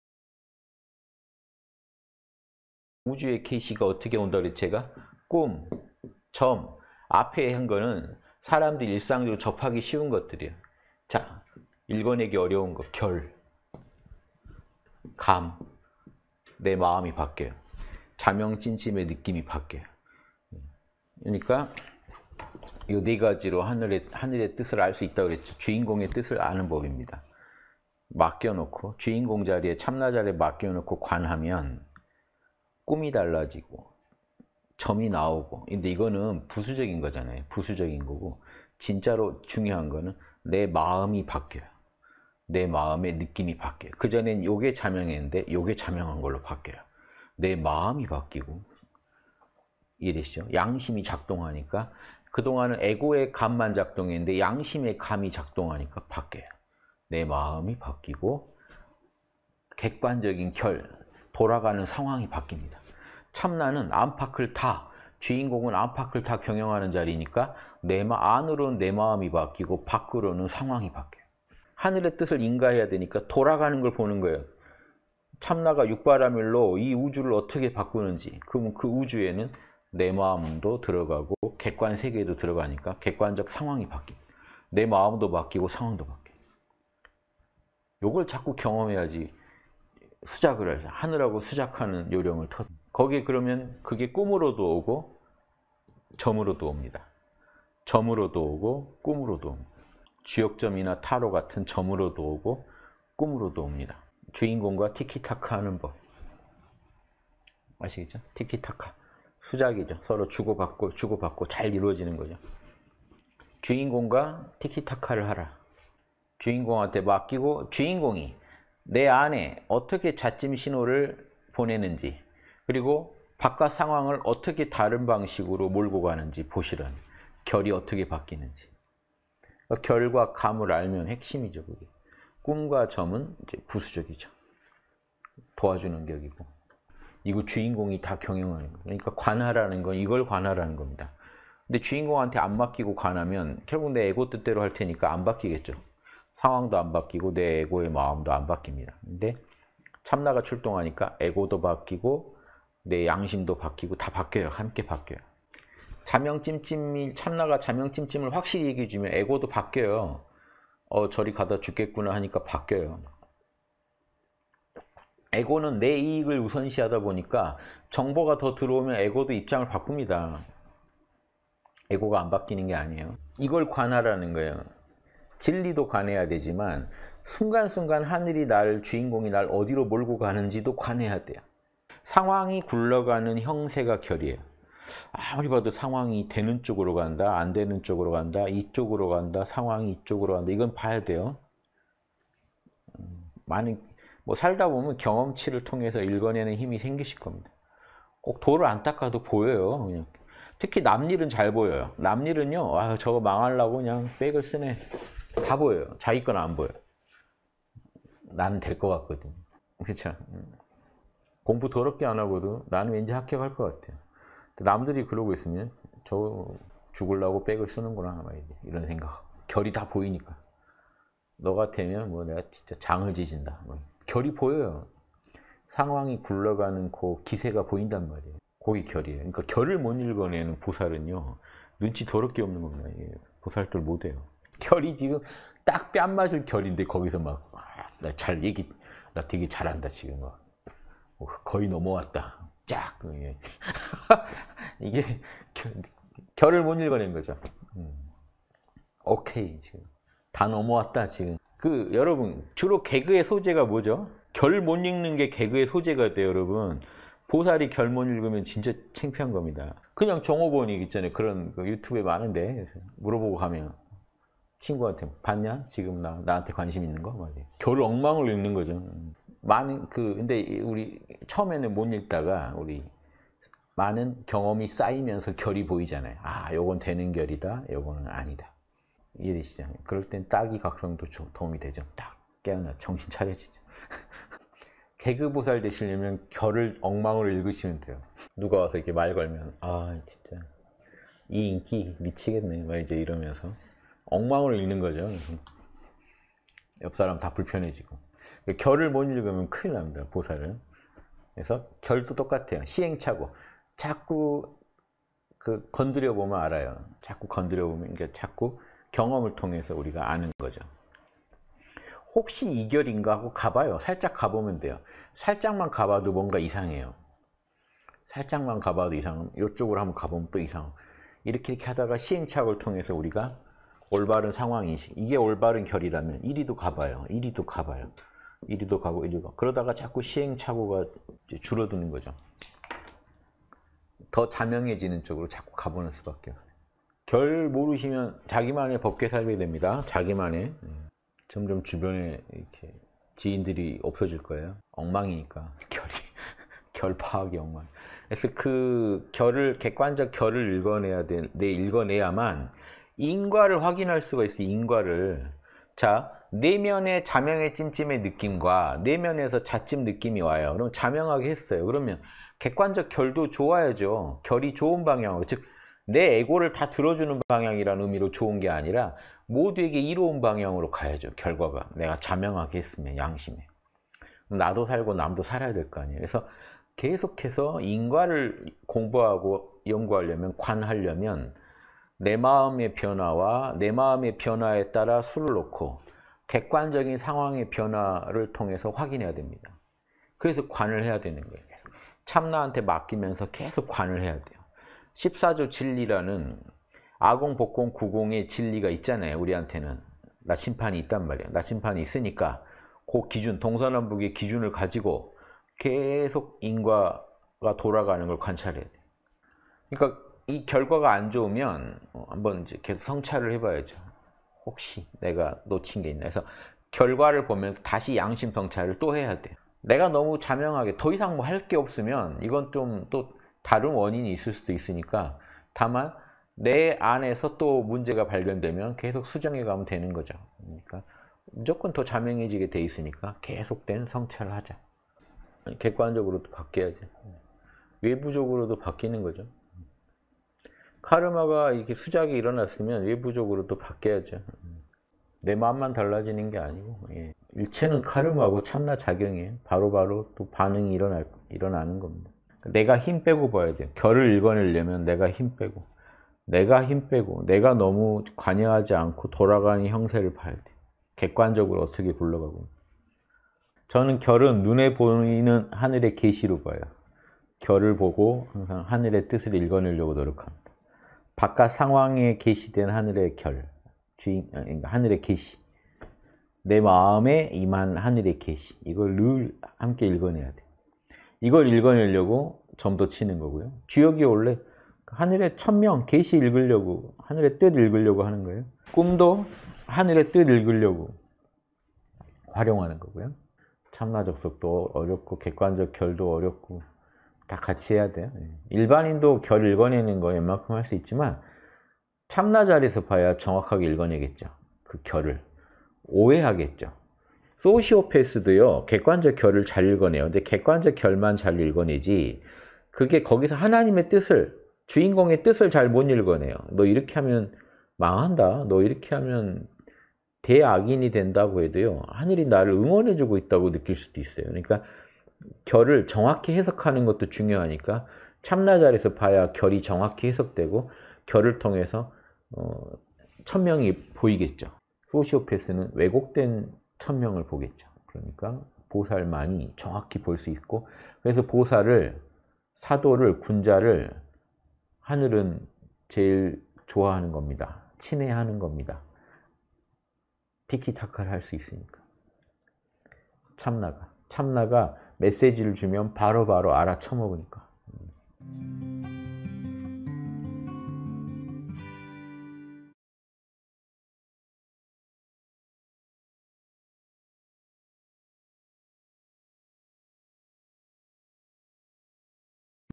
[홍익학당] 현실의 결: 드러난 하늘의 계시
우주의 계시가 어떻게 온다고 그랬죠, 제가?
'꿈', '점(占)'. 앞에 한 거는 사람들이 일상적으로
접하기 쉬운 것들이에요.

자, 읽어내기 어려운 거는 '결', '감'. 내 마음이 바뀌어요.
자명·찜찜의 느낌이 바뀌어요.

그러니까 이거 네 가지로 하늘의 뜻을 알 수 있다고
그랬죠? 주인공의 뜻을 아는 법입니다.

맡겨놓고, 주인공 자리에 참나 자리에 맡겨놓고 관(觀)하면,
'꿈'이 달라지고, '점(占)'이 나오고.

그런데 이거는 부수적인 거잖아요. 부수적인 거고.
진짜로 중요한 거는 내 마음이 바뀌어요.
내 마음의 느낌이 바뀌어요.

그전엔 이게 자명했는데 이게 자명한 걸로 바뀌어요.
내 마음이 바뀌고. 이해되시죠?

양심이 작동하니까 그동안은 에고의 '감'만 작동했는데
양심의 '감'이 작동하니까 바뀌어요.

내 마음이 바뀌고, 객관적인 '결',
돌아가는 상황이 바뀝니다.

참나는 안팎을 다, 주인공은 안팎을 다 경영하는 자리니까
안으로는 내 마음이 바뀌고, 밖으로는 상황이 바뀌어요.

l
상황이 너무 힘들어 죽겠을 때,
'주인공!'하고 관(觀)을 했더니, '주인공!'하고 맡기고 접속,

인가, 14조의 진리 아공·법공·구공의 진리를 인가하면서,
또 뭘 인가해요?

하늘의 뜻을 인가해야 하니까,
돌아가는 걸 보는 거예요.

참나가 6바라밀로 이 우주를 어떻게 바꾸는지,
그러면 그 우주에는 내 마음도 들어가고,
객관 세계도 들어가니까 객관적 상황이 바뀌어요.

내 마음도 바뀌고 상황도 바뀌어요.
이걸 자꾸 경험해야지 수작을 하죠.

하늘하고 수작하는 요령을 터득하죠.
거기에 그러면 그게 '꿈'으로도 오고, '점'으로도 옵니다.

'점'으로도 오고, '꿈'으로도 (오고).
주역점이나 타로 같은 '점'으로도 오고, '꿈'으로도 옵니다.

주인공과 티키타카 하는 법, 아시겠죠?
티키타카, 수작이죠.

서로 주고받고 주고받고, 잘 이루어지는 거죠.
주인공과 티키타카를 하라! 주인공한테 맡기고,

주인공이 내 안에 어떻게 자·찜 신호를 보내는지,
그리고 바깥 상황을 어떻게 다른 방식으로 몰고 가는지
보시라! '결'이 어떻게 바뀌는지.

'결'과 '감'을 알면 핵심이죠, 그게.
'꿈'과 '점'은 이제 부수적이죠. 도와주는 격이고.

객관적으로 상황이 바뀌어야 해요.
상황이에요. '결'은 상황이 바뀌어야 해요.

상황 돌아가는 게 '결'이잖아요?
상황 돌아가는 게 '결'이다. 상황 돌아가는 게 바뀐다.

안으로는 내 마음의 자명·찜찜의 느낌이 바뀐다.
이게 옳다고 느끼다가 이게 옳은 걸로 바뀐다.

이거 주인공이 다 경영하는 거예요.
그러니까 관(觀)하라는 건 이걸 관(觀)하라는 겁니다.

그런데 주인공한테 안 맡기고 관(觀)하면,
결국 내 에고 뜻대로 할 테니까 안 바뀌겠죠?
상황도 안 바뀌고, 내 에고의 마음도 안 바뀝니다.

근데 참나가 출동하니까 에고도 바뀌고,
내 양심도 바뀌고 다 바뀌어요. 함께 바뀌어요.

참나가 자명·찜찜을 확실히 얘기해 주면 에고도 바뀌어요.
'어, 저리 가다가 죽겠구나!'하니까 바뀌어요.

에고는 내 이익을 우선시하다 보니까
정보가 더 들어오면 에고도 입장을 바꿉니다.
에고가 안 바뀌는 게 아니에요. 이걸 관(觀)하라는 거예요.

진리도 관해야 하지만, 순간순간 하늘이 나를,
주인공이 나를 어디로 몰고 가는지도 관해야 해요.

그게 자명이죠. 아무튼 항상 자명해야 해요.
그게 다 '반야(般若)'란 말이에요, '반야'.

접속이 '선정'이라면, 지켜보는 건 '반야'란 말이에요.
반야가 있으면 나머지 6바라밀이 이제 방향을 잡고
출동합니다.

그 방향으로, 이제 나머지 바라밀들이
출동하게 돼 있어요.

그럼 그게 진짜 '맡김'이죠, 이제.
방향까지 나왔으면 거기다 모든 걸 거는 게 '맡김'이다.
그래서 '접속·인가·맡김' 안에 다 들어있다!

'결'과 '감'이 중요하다, '결'을 볼 줄 알아야 해요.

상황이 굴러가는 형세가 '결'이에요.

아무리 봐도 상황이 되는 쪽으로 간다.
안 되는 쪽으로 간다. 이쪽으로 간다.
상황이 이쪽으로 간다. 이건 봐야 해요.

많이 살다 보면 경험치를 통해서 읽어내는 힘이
생기실 겁니다. 꼭 도를 안 닦아도 보여요.

특히 남 일은 잘 보여요. 남 일은요,
'아유, 저거 망하려고 그냥 백을 쓰네!' 다 보여요.

자기 건 안 보여요. 나는 될 것 같거든요. 그렇죠?
그 공부 더럽게 안 하고도 나는 왠지 합격할 것 같아요.

남들이 그러고 있으면 '저거 죽으려고 백을 쓰는구나!'
이런 생각하고, 결이 다 보이니까.

'네가 되면 뭐 내가 진짜 장을 지진다!' 결이 보여요.
상황이 굴러가는 그 기세가 보인단 말이에요.
그게 '결'이에요.

그러니까 '결'을 못 읽어내는 보살은요,
눈치 더럽게 없는 겁니다. 보살도를 못 해요.

'결'이 지금 딱 뺨 맞을 결인데 거기서 막
"나 얘기 되게 잘한다. 지금 막 거의 넘어왔다."

"짝!" 그러면 이게 '결'을 못 읽어낸 거죠.
"OK! 지금 다 넘어왔다, 지금."

여러분, 주로 개그의 소재가 뭐죠?
'결'을 못 읽는 게 개그의 소재가 돼요, 여러분.
보살이 '결' 못 읽으면 진짜 창피한 겁니다.

그냥 종업원이 있잖아요 (그런 유튜브에 많은데)
(누가) 물어보고 가면, 친구한테

"봤냐? 지금 나한테 관심 있는 거"
결을 엉망으로 읽는 거죠.

그런데 우리가 처음에는 못 읽다가
많은 경험이 쌓이면서 '결'이 보이잖아요?
아! 이건 되는 '결'이다, 이거는 아니다. 이해되시죠?

그럴 때는 따귀 각성도 도움이 되죠.
"짝!" 깨어나 정신 차려지죠.
개그 보살 되시려면 결을 엉망으로 읽으시면 돼요.

누가 와서 이렇게 말 걸면, "아! 진짜 이 인기 미치겠네!"
막 이제 이러면서, 엉망으로 읽는 거죠.

그래서 옆 사람 다 불편해지고.
'결'을 못 읽으면 큰일 납니다, 보살은.

그래서 '결'도 똑같아요. 시행착오,
자꾸 건드려보면 알아요. 자꾸 건드려보면,
자꾸 경험을 통해서 우리가 아는 거죠.

'혹시 이 '결'인가?'하고 가봐요. 살짝 가보면 돼요.
살짝만 가봐도 뭔가 이상해요.

살짝만 가봐도 이상하면, 이쪽으로 한번 가보면
또 이상하고.

이렇게 이렇게 하다가 시행착오를 통해서 우리가
올바른 상황 인식(을 해요).

이게 올바른 결이라면 이리도 가봐요.
이리도 가봐요. 이리도 가고, 이리도 가요.

그러다가 자꾸 시행착오가 줄어드는 거죠.
더 자명해지는 쪽으로 자꾸 가보는 수밖에 없어요.

'결'을 모르시면 자기만의 법계에 살게 됩니다, 자기만의.
점점 주변에 이렇게 지인들이 없어질 거예요.
엉망이니까 '결'이. '결' 파악이 엉망.

그래서 그 '결'을, 객관적 '결'을 읽어내야만
인과를 확인할 수가 있어요, 인과를.

자, 내면의 자명의, 찜찜의 느낌과
내면에서 자찜 느낌이 와요. 그러면 자명하게 했어요.
그러면 객관적 결도 좋아야죠?

결이 좋은 방향으로, 즉 내 에고를 다 들어주는 방향이라는
의미로 좋은 게 아니라

모두에게 이로운 방향으로 가야죠, 결과가.
내가 자명하게 했으면, 양심에.
그럼 나도 살고 남도 살아야 할 거 아니에요.

그래서 계속해서 인과를 공부하고 연구하려면, 관하려면
내 마음의 변화와 내 마음의 변화에 따라 수를 놓고,

객관적인 상황의 변화를 통해서 확인해야 합니다.
그래서 관(觀)을 해야 하는 거예요.

참나한테 맡기면서 계속 관을 해야 해요.
'14조 진리'라는, 아공·법공·구공의 진리가 있잖아요,
우리한테는. 나침판이 있단 말이에요.

나침판이 있으니까, 그 기준 동서남북의 기준을 가지고
계속 인과가 돌아가는 걸 관찰해야 해요.

그러니까 이 결과가 안 좋으면
한번 계속 성찰을 해봐야죠.

'혹시, 내가 놓친 게 있나?'해서 결과를 보면서
다시 양심성찰을 또 해야 해요.

내가 너무 자명하게, 더 이상 뭐 할 게 없으면
이건 좀 또 다른 원인이 있을 수도 있으니까

다만, 내 안에서 또 문제가 발견되면
계속 수정해가면 되는 거죠.

그러니까 무조건 더 자명해지게 돼 있으니까
계속된 성찰을 하자!

비슷한 상황이 살면서 계속 일어나는데, 이 상황을 대하는
내 마음가짐과 행동이 달라지는 걸 느낍니다.

'결'이 바뀐다는 게, 상황 자체가
외부적으로 바뀌는 게 아니라

내가 내 마음 가지고 상황을 6바라밀에 돌려버리니
결과가 달라지더라 하는 거 배워가는 거겠죠?

객관적으로도 바뀌어야지 외부적으로도 바뀌는 거죠.
카르마가 이렇게 수작이 일어났으면
외부적으로도 바뀌어야죠.

내 마음만 달라지는 게 아니고요, 네.
일체는 카르마고 참나 작용이에요.
바로바로 또 반응이 일어나는 겁니다.

내가 힘 빼고 봐야 해요.
'결'을 읽어내려면 내가 힘 빼고.

(후략)